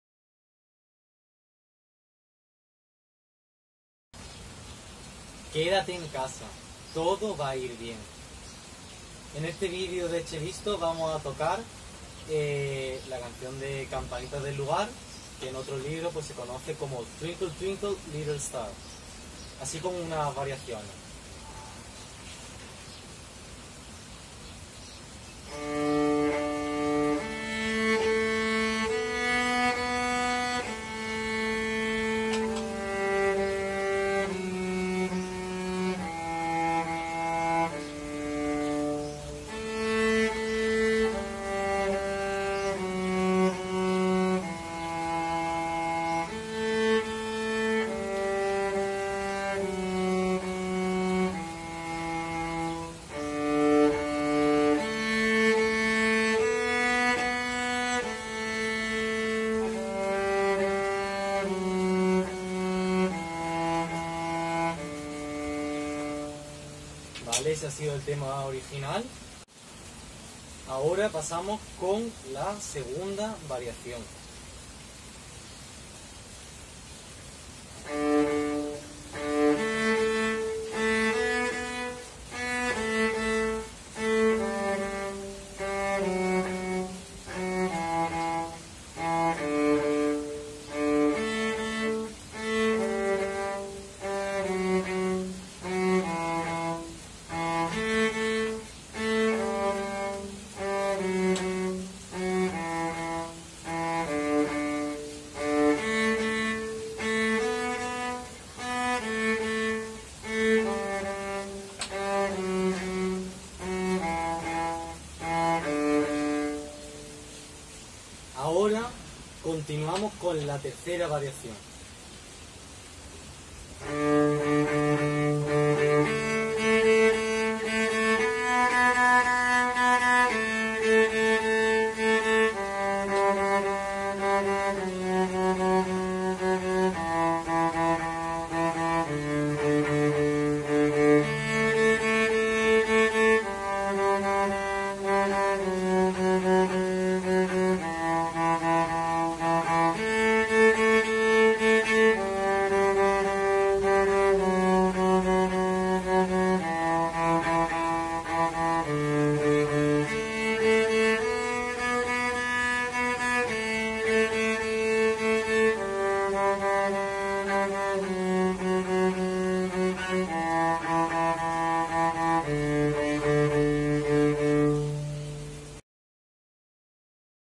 🎻⚠️☣MÚSICA VIRAL (CONFINAVIRUS) ► SEGUNDO DÍA de CONFINAMIENTO CORONAVIRUS | Día 02 CELLISTO
[APOYO MUSICAL]
Hola, muy buenas a tod@s,

Desde Cellisto, subimos el vídeo de este "segundo" día (segundo día tocando) de cuarentena debido al coronavirus, COVID-19. En él, veremos y escucharemos una canción muy conocida. En concreto, el 'Tema Base' y 2 variaciones (la 2ª y 3ª, ya que la Plataforma ha decidido prescindir de las variaciones 1 y 4…).

Considero necesaria esta iniciativa en la que nosotros, los MÚSICOS, podemos aportar nuestro ‘granito de arena’ para actuar como unidad y tranquilizar, así como apoyar de manera solidaria, a todas las personas en estos difíciles momentos.

Cellisto Music Learning

Lo dicho, no soy el propietario ni compositor de la obra, simplemente me dedico a interpretarla y mostrárosla a vosotros, el público.